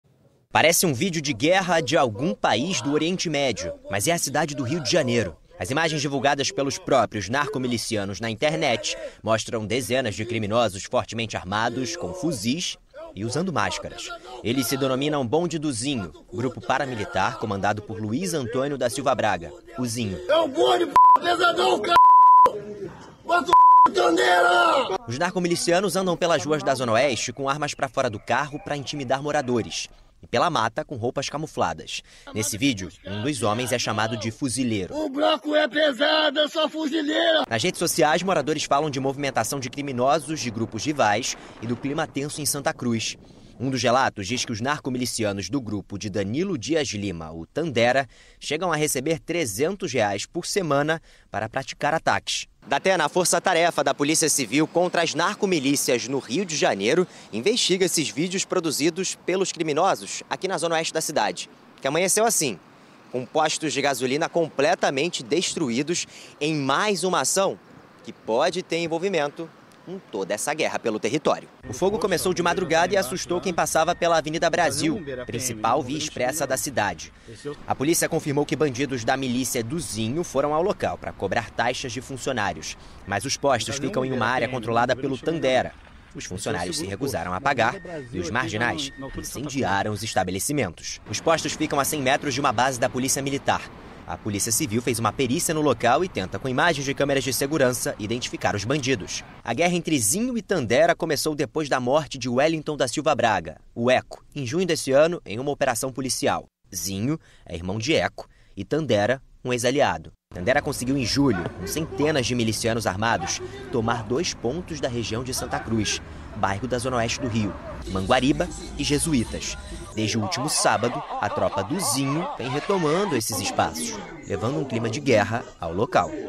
GUERRA ENTRE MILÍCIAS NO RJ
Dois postos são queimados durante confronto